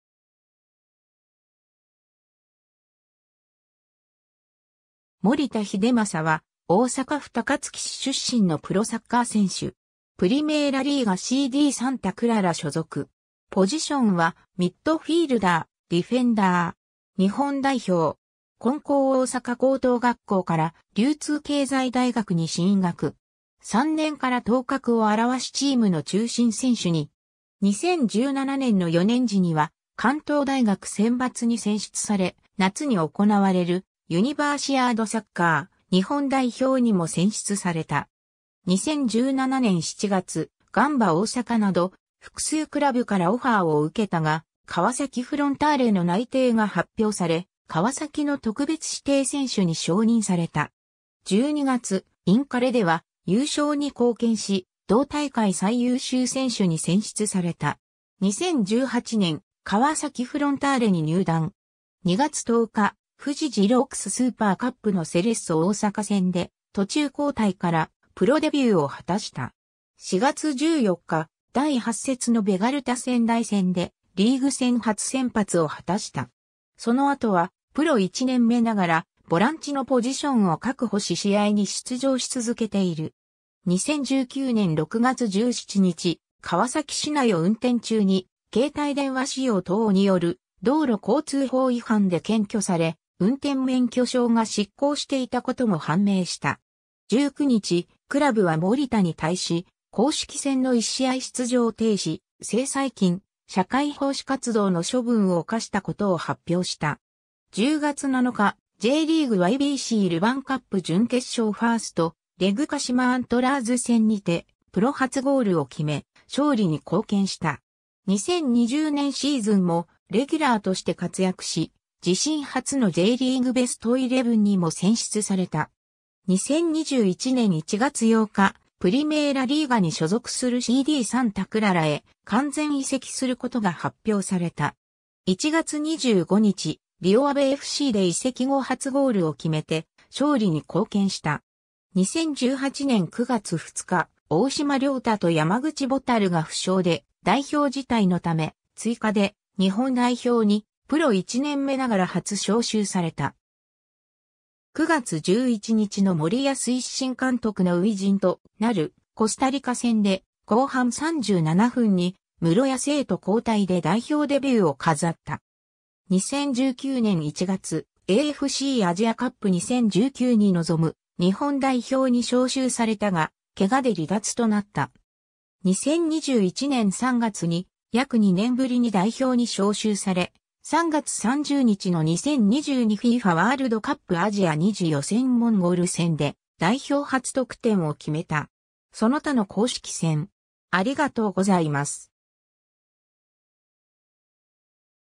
守田英正
CDサンタ・クララの選手
守田 英正（もりた ひでまさ、1995年5月10日 - ）は、大阪府高槻市出身のプロサッカー選手。
プリメイラ・リーガ・CDサンタ・クララ所属。
ポジションはミッドフィールダー、ディフェンダー。
日本代表。
 金光大阪高等学校から流通経済大学に進学。
3年から頭角を現しチームの中心選手に。
2017年の4年時には関東大学選抜に選出され、夏に行われるユニバーシアードサッカー日本代表にも選出された。
2017年7月、ガンバ大阪など複数クラブからオファーを受けたが、川崎フロンターレの内定が発表され、川崎の特別指定選手に承認された。
12月、インカレでは優勝に貢献し、同大会最優秀選手に選出された。
 2018年、川崎フロンターレに入団。
2月10日、FUJI XEROX SUPER CUPのセレッソ大阪戦で途中交代からプロデビューを果たした。
4月14日、第8節のベガルタ仙台戦でリーグ戦初先発を果たした。
その後はプロ1年目ながらボランチのポジションを確保し試合に出場し続けている。
 2019年6月17日、川崎市内を運転中に「携帯電話使用等」による道路交通法違反で検挙され、運転免許証が失効していたことも判明した。
19日、クラブは守田に対し、公式戦の1試合出場停止、制裁金、社会奉仕活動の処分を科したことを発表した。
10月7日、JリーグYBCルヴァンカップ準決勝1stレグ鹿島アントラーズ戦にてプロ初ゴールを決め、勝利に貢献した。
 2020年シーズンもレギュラーとして活躍し、自身初のJリーグベストイレブンにも選出された。
 2021年1月8日、プリメイラ・リーガ（ポルトガル1部）に所属するCDサンタ・クララへ完全移籍する事が発表された。
1月25日、リオ・アヴェFCで移籍後初ゴールを決めて勝利に貢献した。
 2018年9月2日、大島僚太と山口蛍が負傷で代表辞退のため、追加で日本代表にプロ一年目ながら初招集された。
9月11日の森保一新監督の初陣となるコスタリカ戦で後半37分に室屋成と交代で代表デビューを飾った。
 2019年1月、AFCアジアカップ2019に臨む日本代表に招集されたが、怪我で離脱となった。
 2021年3月に約2年ぶりに代表に招集され、3月30日の2022 FIFAワールドカップ・アジア2次予選モンゴル戦で代表初得点を決めた。
 その他の公式戦